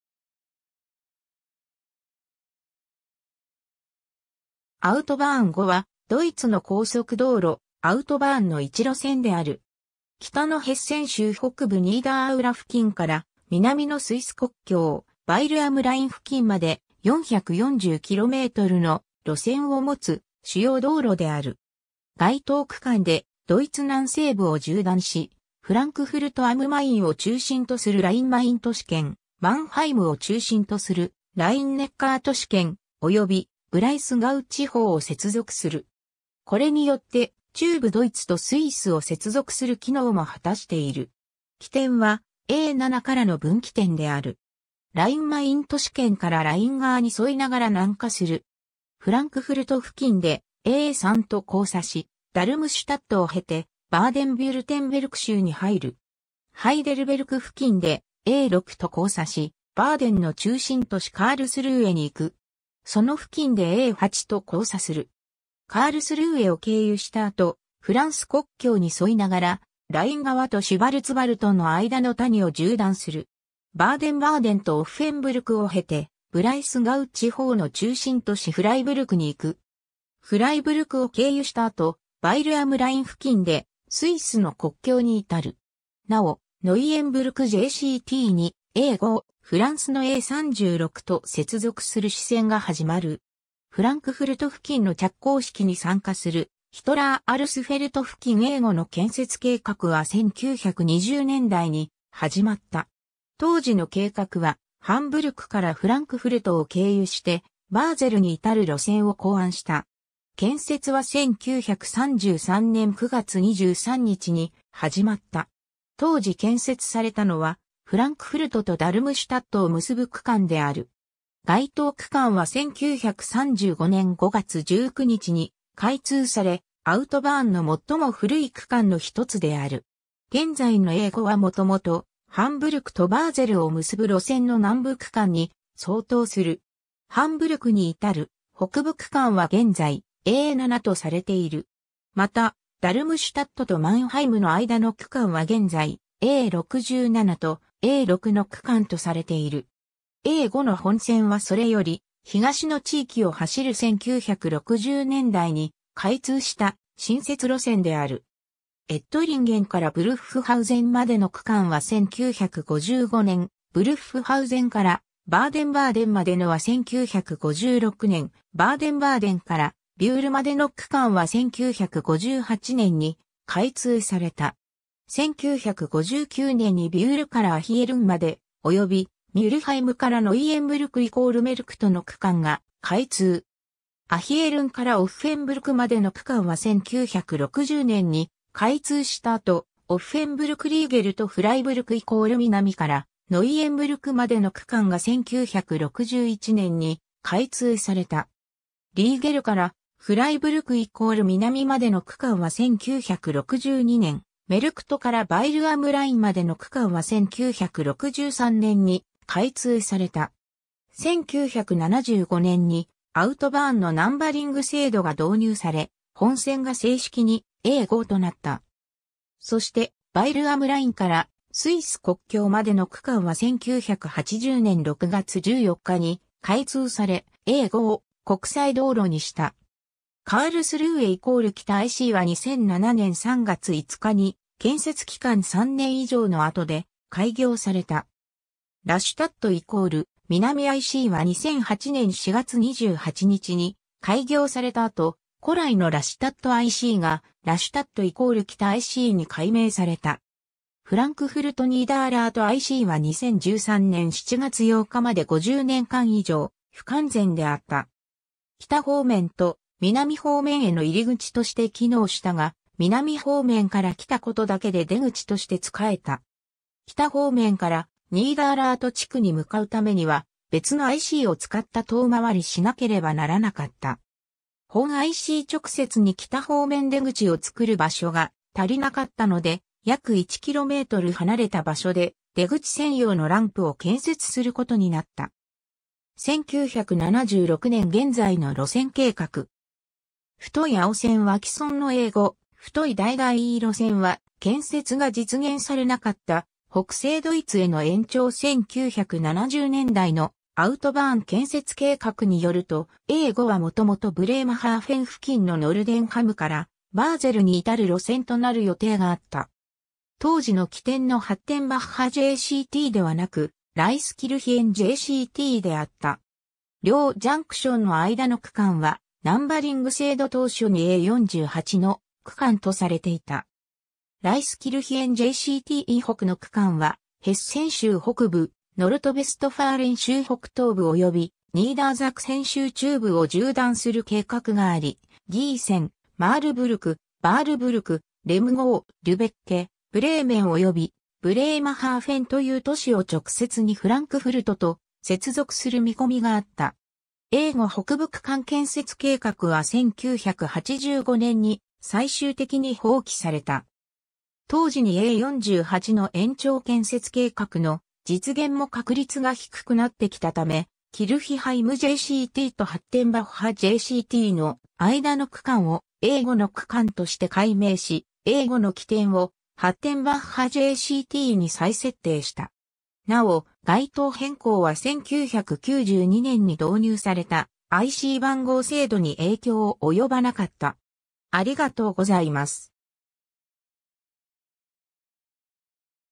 アウトバーン 5
バーデン＝ヴュルテンベルクの交通
アウトバーン 5（ドイツ語: Bundesautobahn 5, BAB 5 または A 5）はドイツの高速道路、アウトバーンの一路線である。
 北のヘッセン州北部ニーダーアウラ付近から南のスイス国境、ヴァイル・アム・ライン付近まで、440 kmの路線を持つ主要道路である。
該当区間でドイツ南西部を縦断し、フランクフルト・アム・マインを中心とするライン・マイン都市圏、マンハイムを中心とするライン・ネッカー都市圏、およびブライスガウ地方を接続する。
これによって中部ドイツとスイスを接続する機能も果たしている。
 起点はA7からの分岐点である。
ライン・マイン都市圏からライン川に沿いながら南下する。
フランクフルト付近でA3と交差し、ダルムシュタットを経てバーデン＝ヴュルテンベルク州に入る。
ハイデルベルク付近でA6と交差し、バーデンの中心都市カールスルーエに行く。
その付近でA8と交差する。
カールスルーエを経由した後、フランス国境に沿いながら、ライン川とシュヴァルツヴァルトの間の谷を縦断する。
バーデン＝バーデンとオッフェンブルクを経て、ブライスガウ地方の中心都市フライブルクに行く。
フライブルクを経由した後、ヴァイル・アム・ライン付近でスイスの国境に至る。
なお、ノイエンブルクJCTにA5をフランスのA 36と接続する支線が始まる。
 フランクフルト付近の着工式に参加するヒトラー アルスフェルト付近（1949年） A 5の建設計画は1920年代に始まった。
当時の計画はハンブルクからフランクフルトを経由してバーゼルに至る路線を考案した（HaFraBaプロジェクト）。
建設は1933年9月23日に始まった。
当時建設されたのはフランクフルトとダルムシュタットを結ぶ区間である。
該当区間は1935年5月19日に開通され、アウトバーンの最も古い区間の一つである（最古は1932年に開通され、現在A 555の区間であるケルン－ボン線）。
 現在のA 5はもともとハンブルクとバーゼルを結ぶ路線の南部区間に相当する。
ハンブルクに至る北部区間は現在A 7とされている。
また、ダルムシュタットとマンハイムの間の区間は現在A 67とA 6の区間とされている。
A 5の本線はそれより東の地域を走る1960年代に開通した新設路線である。
 エットリンゲンからブルッフハウゼンまでの区間は1955年、ブルッフハウゼンからバーデン＝バーデンまでのは1956年、バーデン＝バーデンからビュールまでの区間は1958年に開通された。
1959年にビュールからアヒェルンまで、およびミュルハイムからノイエンブルク＝メルクトの区間が開通。
アヒェルンからオッフェンブルクまでの区間は1960年に開通した後、オッフェンブルク＝リーゲルとフライブルク＝南からノイエンブルクまでの区間が1961年に開通された。
リーゲルからフライブルク＝南までの区間は1962年、メルクトからヴァイル・アム・ラインまでの区間は1963年に開通された。
1975年にアウトバーンのナンバリング制度が導入され、本線が正式に「A 5」となった。
そして、ヴァイル・アム・ラインからスイス国境までの区間は1980年6月14日に開通され、A 5を国際道路にした。
 カールスルーエ＝北ICは2007年3月5日に、建設期間3年以上の後で、開業された。
ラシュタット＝南ICは2008年4月28日に開業された後、古来のラシュタットICがラシュタット＝北ICに改名された。
フランクフルト＝ニーダーラートICは2013年7月8日まで50年間以上不完全であった。
北方面と南方面への入口として機能したが、南方面から来たことだけで出口として仕えた。
北方面からニーダーラート地区に向かうためには、別のICを使った遠回りしなければならなかった。
本IC直接に北方面出口を作る場所が足りなかったので、約1km離れた場所で出口専用のランプを建設することになった。
 1976年現在の路線計画。
太い青線は既存のA 5、太い橙色線は建設が実現されなかった北西ドイツへの延長 1970年代のアウトバーン建設計画によると、A 5はもともとブレーマハーフェン付近のノルデンハムからバーゼルに至る路線となる予定があった。
当時の起点のハッテンバッハJCTではなく、ライスキルヒェンJCTであった。
両ジャンクションの間の区間はナンバリング制度当初にA 48の区間とされていた。
ライスキルヒェンJCT以北の区間はヘッセン州...